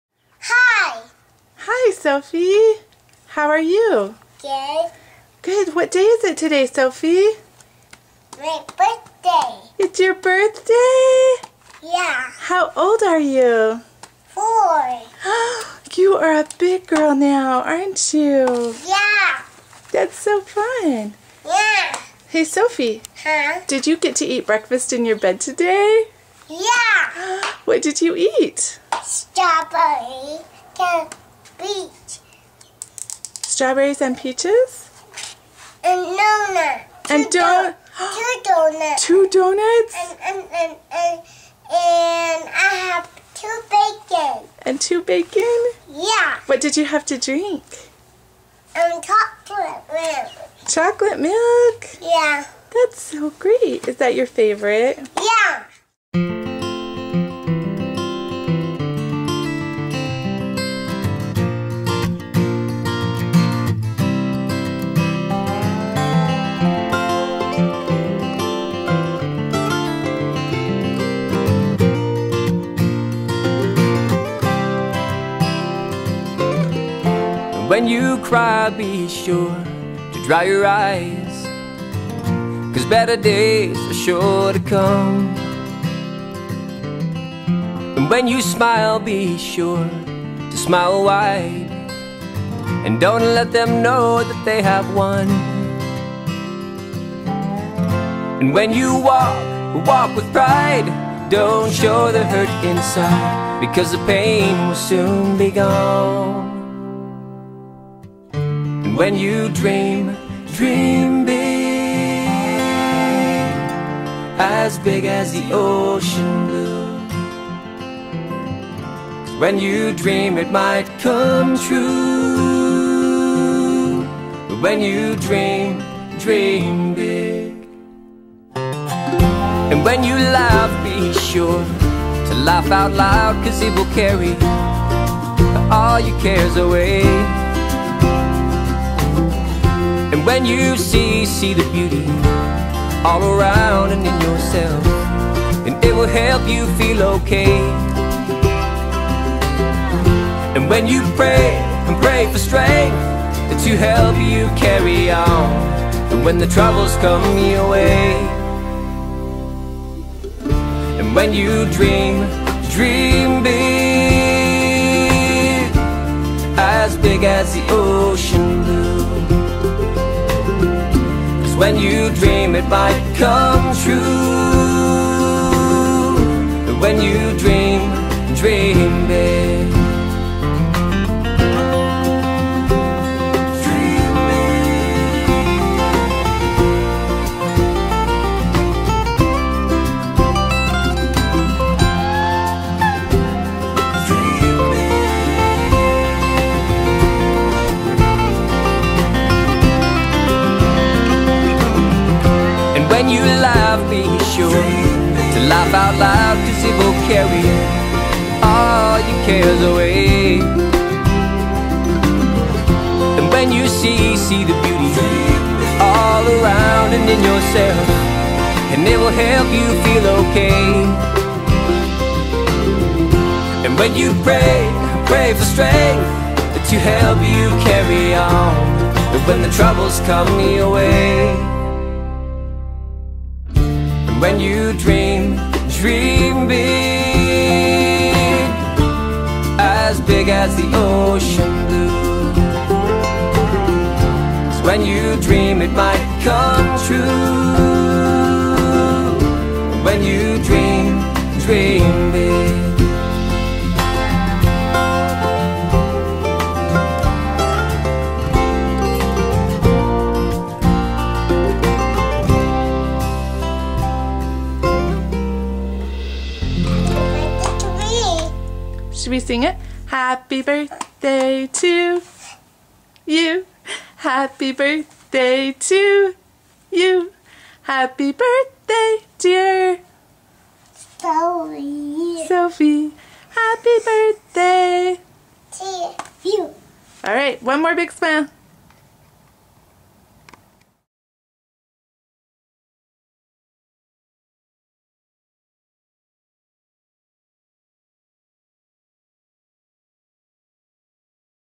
Sophi's 4th Birthday
Our sweet Sophi, home a year and a half from China.  You are so loved, baby girl!  Happy 4th Birthday!!!